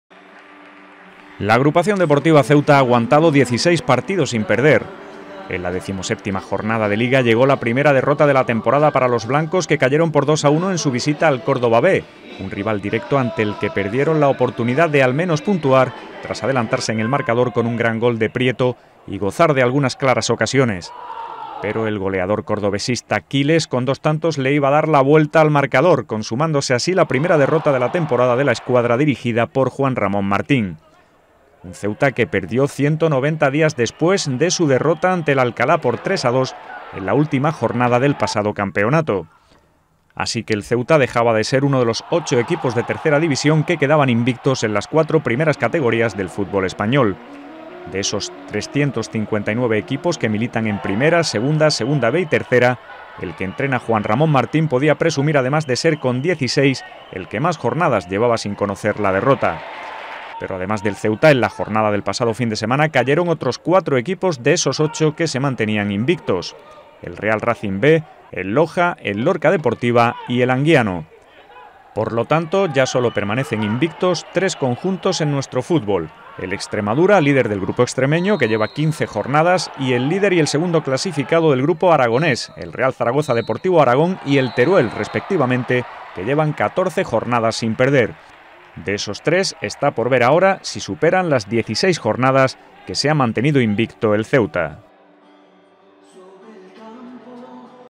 La A.D.Ceuta consiguió estar 16 jornadas invicto
El equipo caballa logra su segundo mejor inicio de temporada de su andadura en tercera división